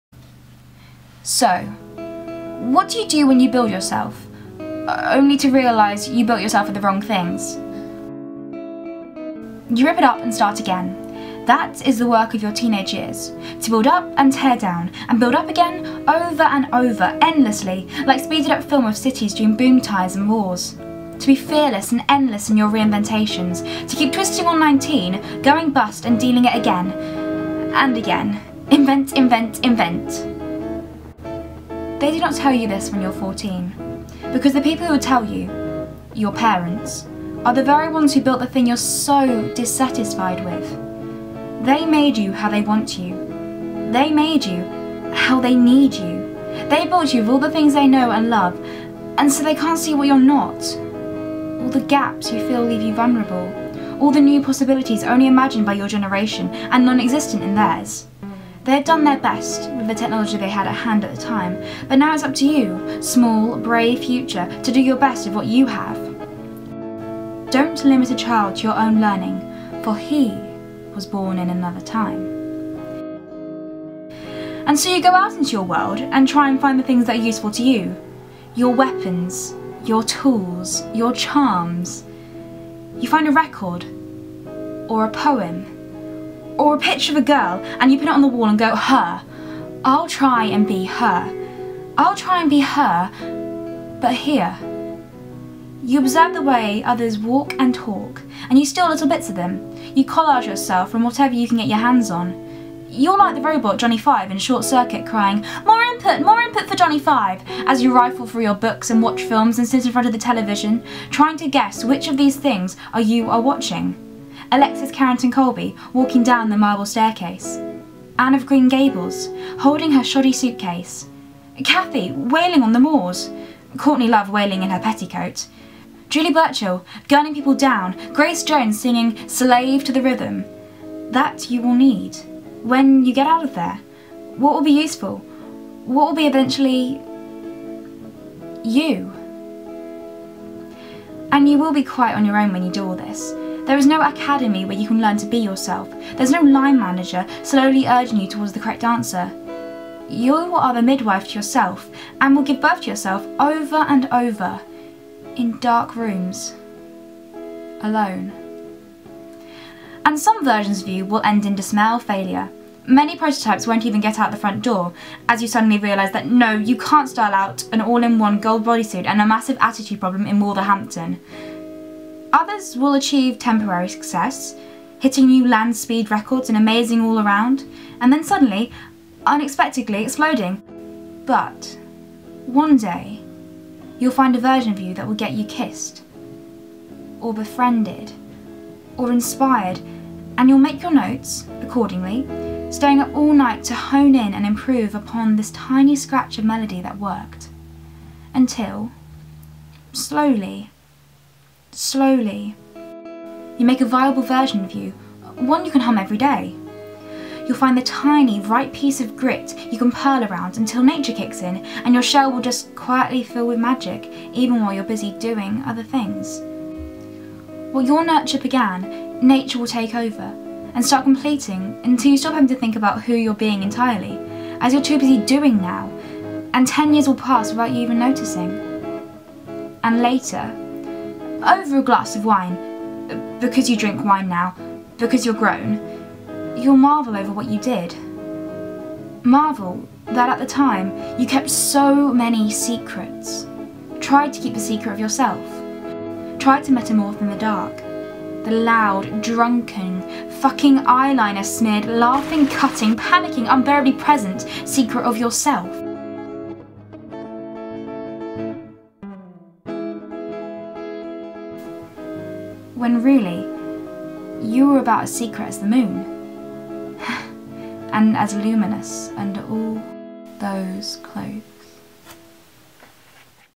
VEDA 1- how to build a girl.
I think the video basically says everything I need it to say!

Hey Everyone, I'm going to try and do VEDA (Vlog EveryDay in August) seeing as this will possibly be the busiest, and maybe most dramatic (I get my AS level results!) month of the year (not sure if thats accurate, but roll with me)

Follow me around before summer ends! :)

I'd also like to mention my friend William! He is the provider of the music you can hear in the background- Thanks Will!! :)